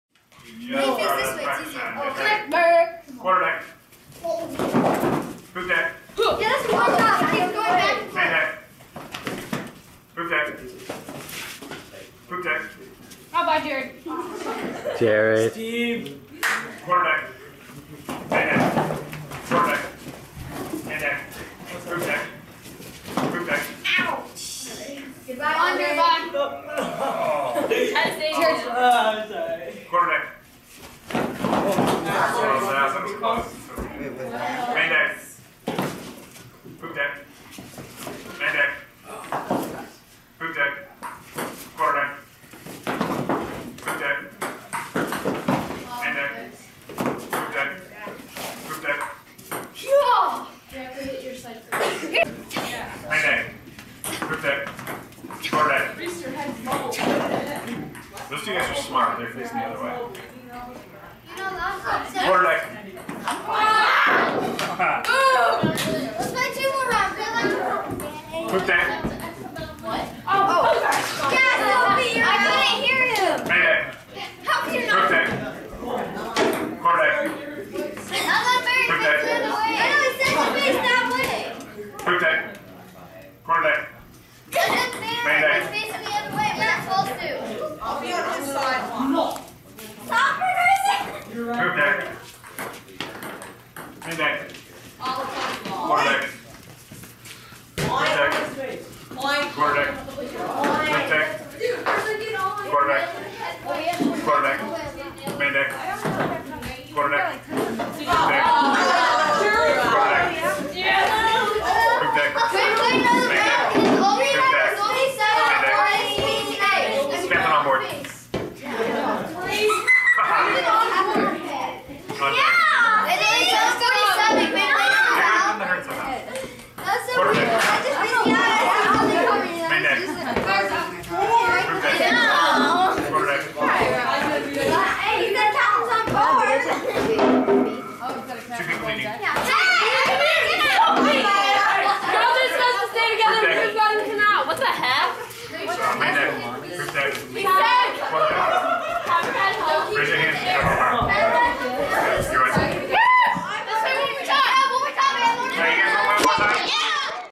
Jr. High - Poop Deck, Main Deck, Quarter Deck - 1-23-13
A popular game at the Wednesday night Jr. High youth group at Kumulani Chapel.